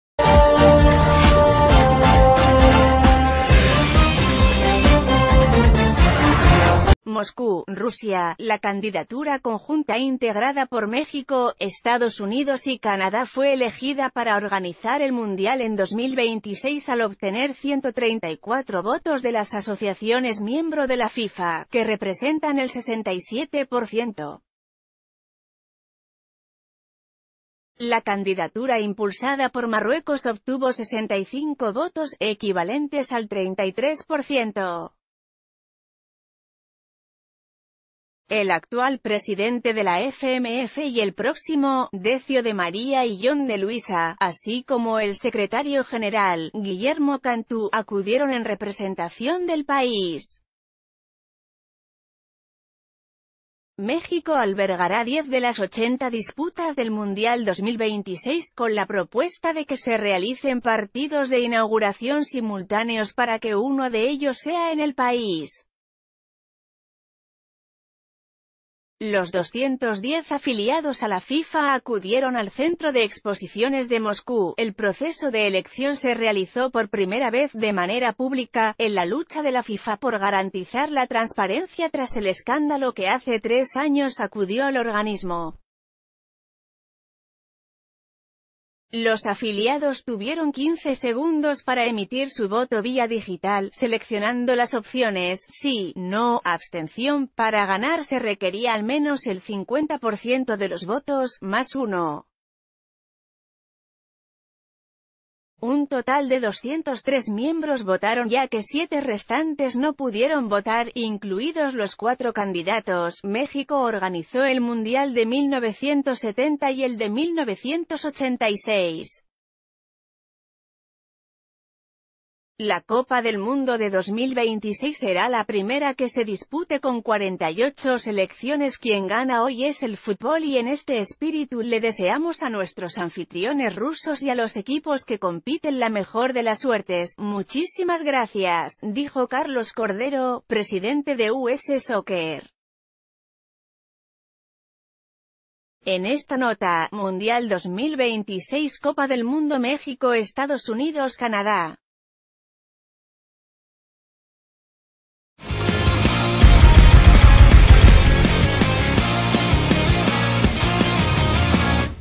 México, Estados Unidos y Canadá, sedes del Mundial 2026
Moscú, Rusia.- La candidatura conjunta integrada por México, Estados Unidos y Canadá fue elegida para organizar el Mundial en 2026 al obtener 134 votos de las asociaciones miembro de la FIFA, que representan el 67 por ciento. La candidatura impulsada por Marruecos obtuvo 65 votos, equivalentes al 33...